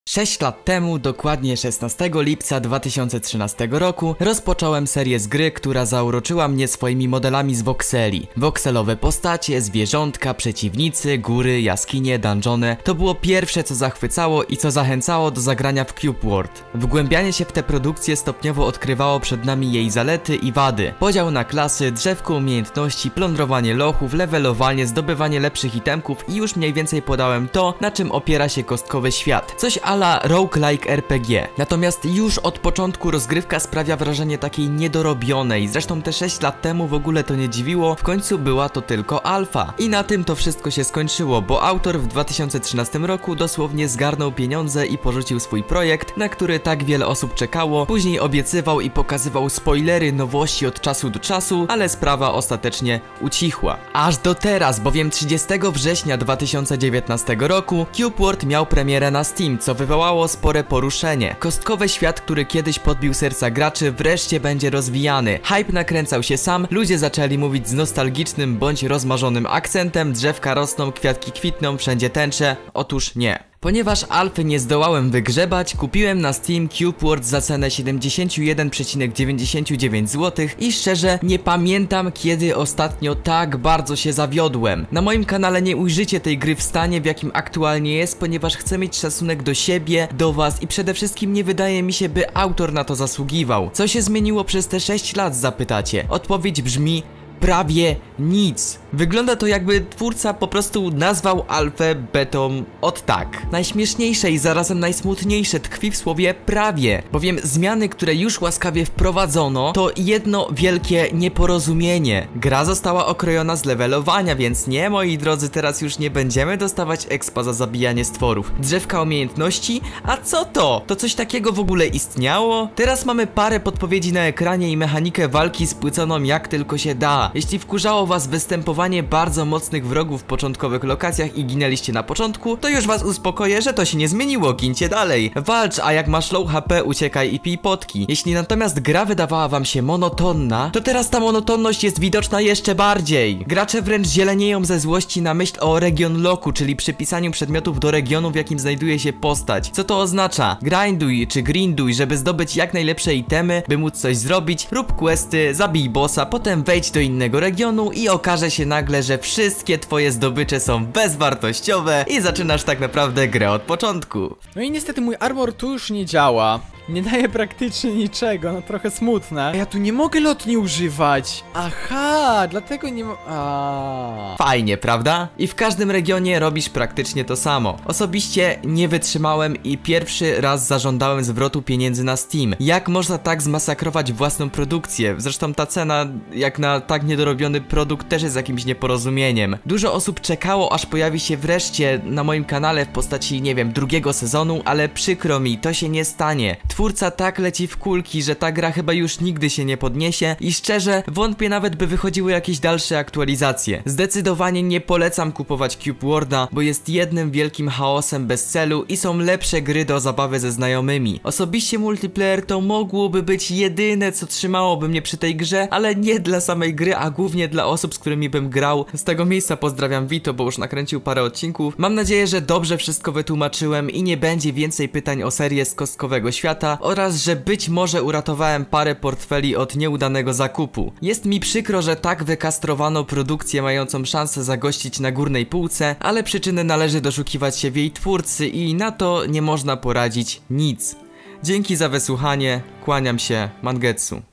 CUBE WORLD 2019, CZYLI PIERWSZY RAZ ŻĄDAŁEM ZWROTU PIENIĘDZY
moja opinia czy tam recenzja i odpowiedź na wszelkie pytania dotyczace serii z tej gry